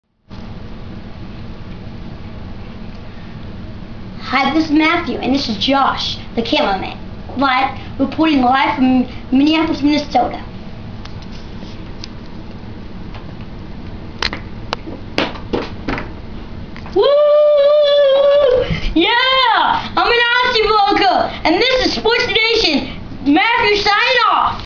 Matthew from Minneapolis with great putt!
I shot this in my basement and it was a lot of fun.  All of the bobbleheads are minnesota sports related- except for the Obama one.